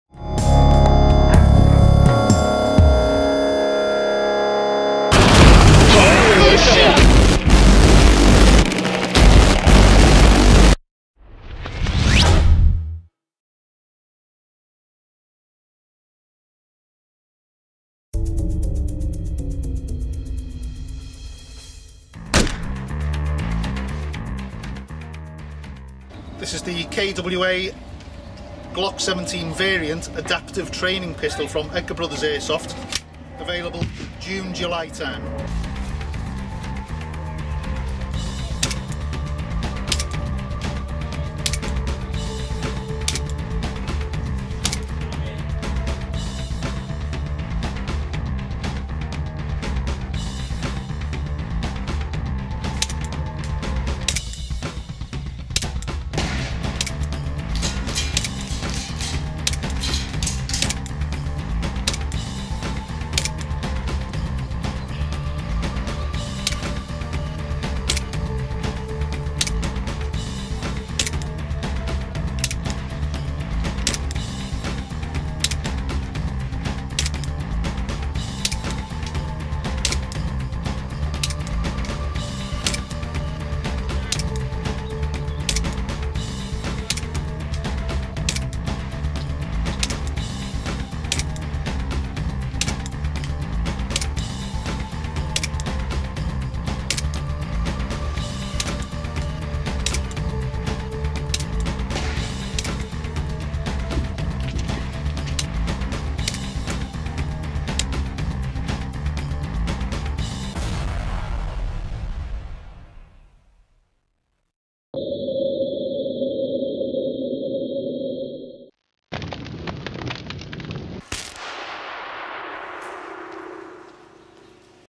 KWA ATP Pistol @ AAF5
Featuring the KWA ATP - a GBB Pistol coming to the UK June/July 2012 authorised KWA distributors Edgar Brothers Airsoft UK
Created By: Yosser @ Airsoft Odyssey

Starring BAMF from The Airsoft Exchange Forums
Filmed on location at Airsoft Arms Fair, The Grange, Frog Lane, Balsall Common West Mildlands UK